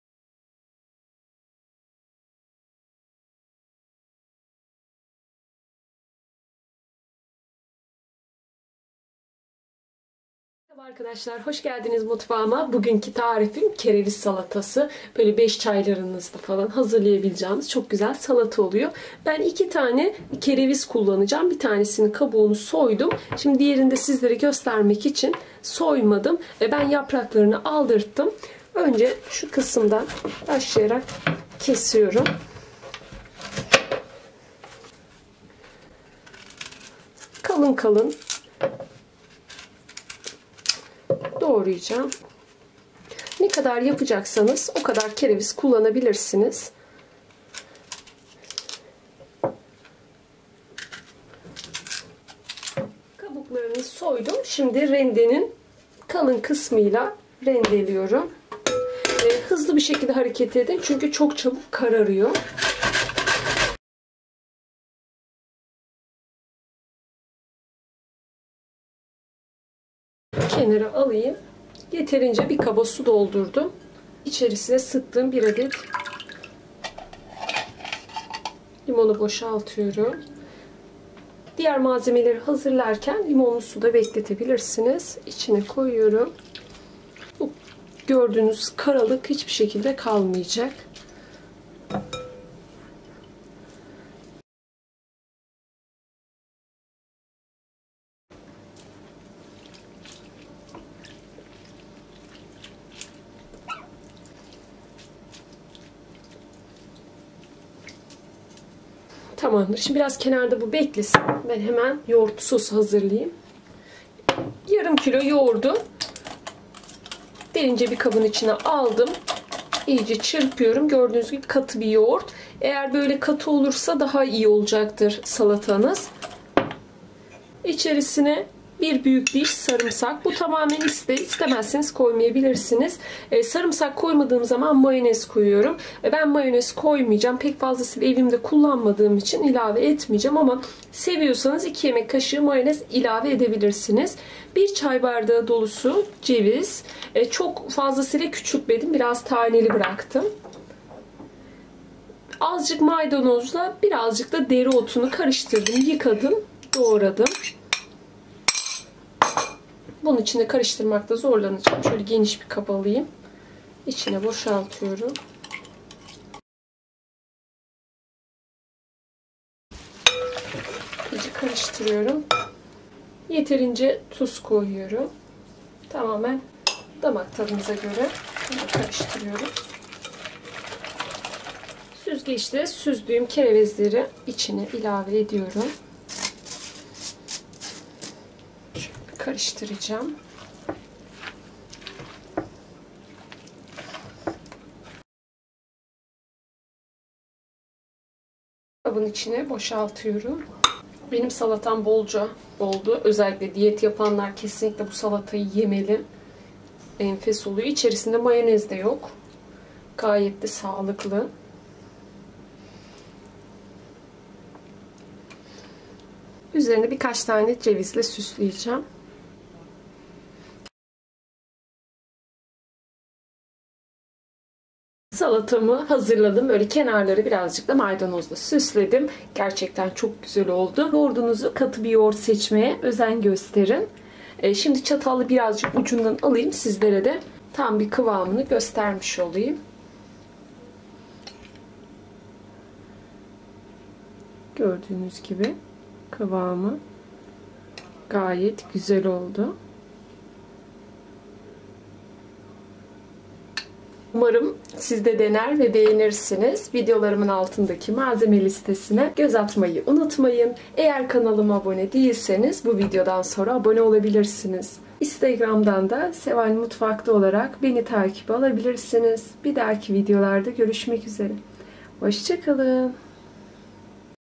Yoğurtlu Salataların En Güzeli En Sağlıklısı En Vitaminlisi Kereviz Salatası 💯Seval Mutfakta
Malzemeler ✔️
2 adet orta boy kereviz
1 adet limonun suyu
1 miktar maydanoz
1 miktar dereotu
İsteğe bağlı 1 diş sarımsak
1 çay bardağı ceviz
Yarım kğ yoğurt kullandım kerevizin miktarına göre değişe bilir
Tuz
❗️İsterseniz 2 yemek kaşığı mayonezde ilave edebilirsiniz
❗️Yoğurdunuzu çok suyu tercih ederseniz sulanma yapacaktır